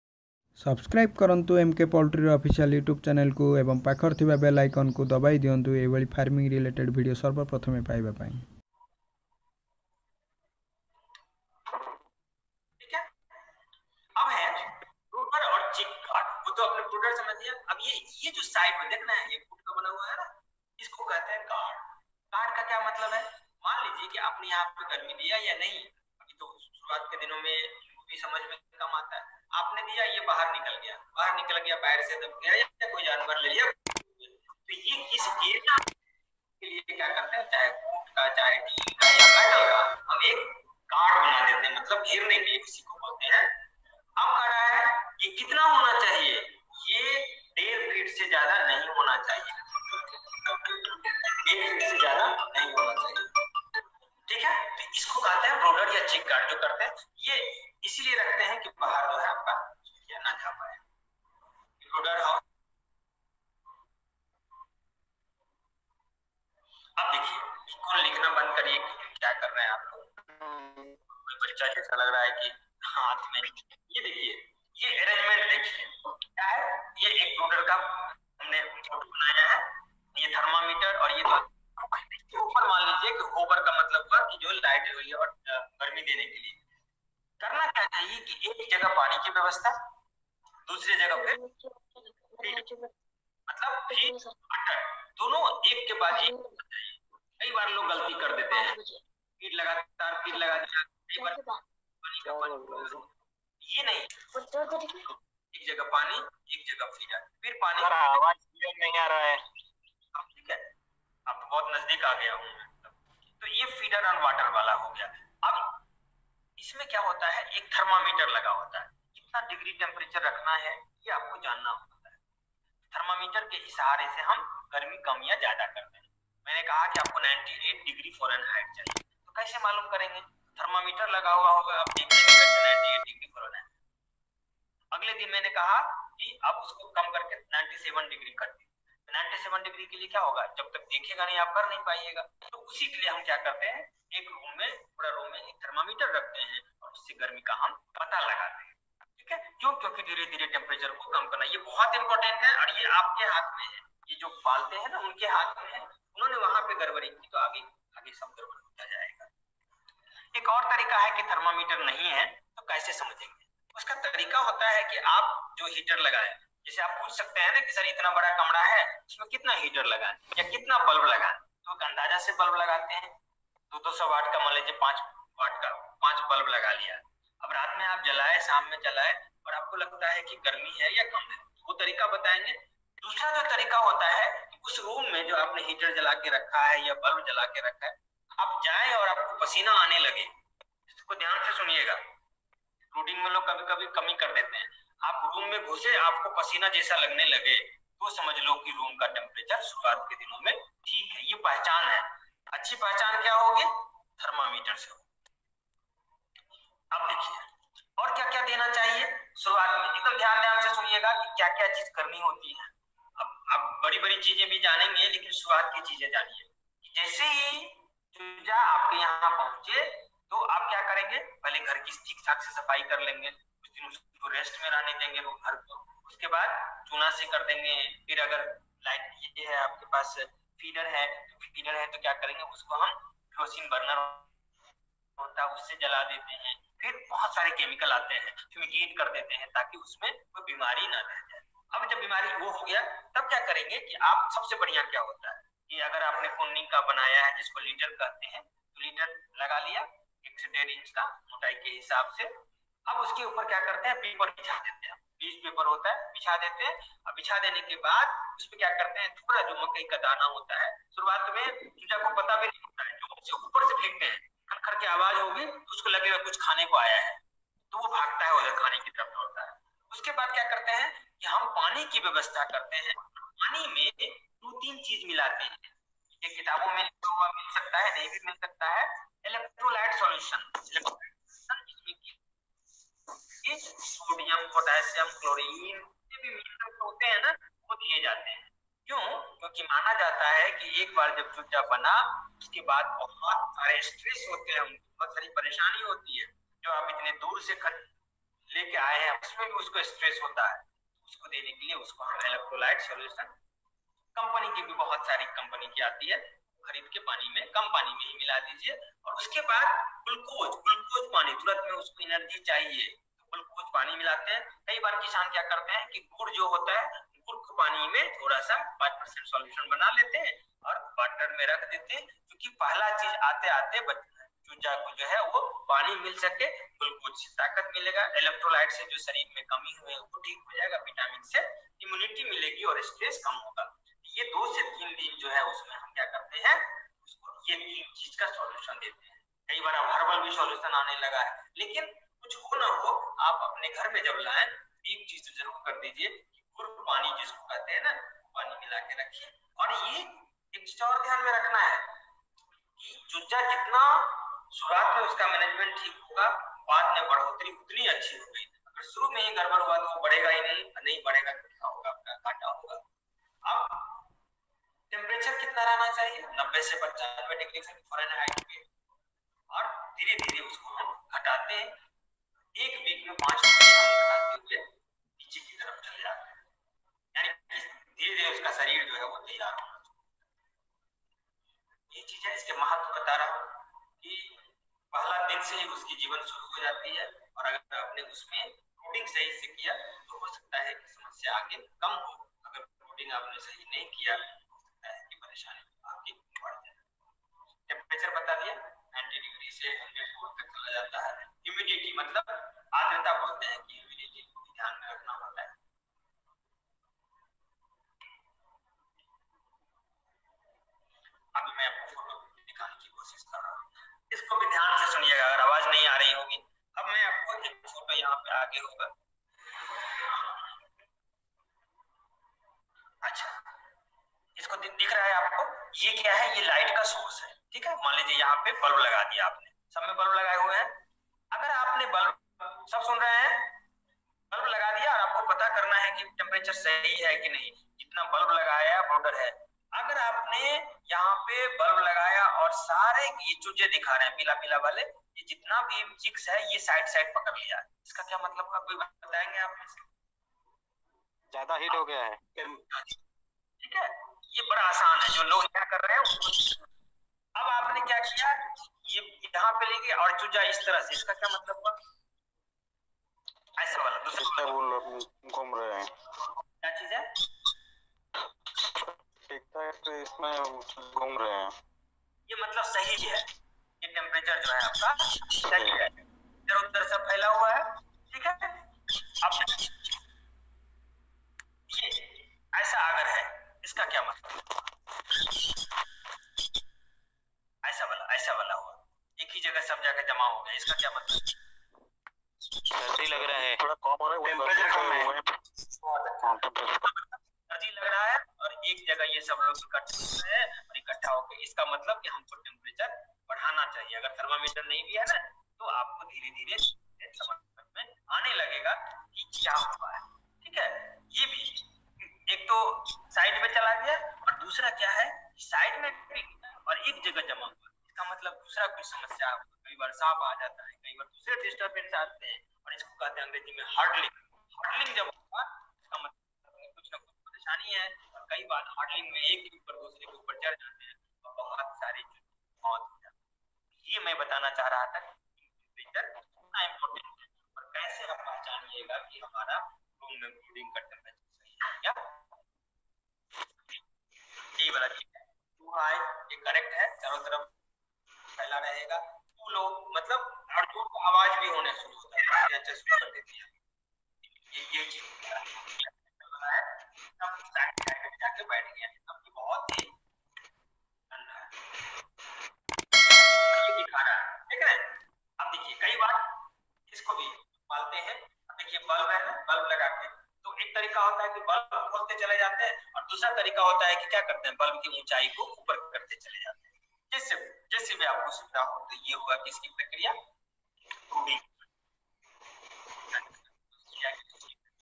Poultry training day-2 ,part-2 this training organized by RDC (Rural Development council)
Hii viewers,
This training organized by RDC
Please like and follow our official Facebook, Instagram, Twitter pages for more update our products booking date,chicks and other🙏🐣🙏

Instagram-I'm on Instagram as @m.k.poultry. Install the app to follow my photos and videos.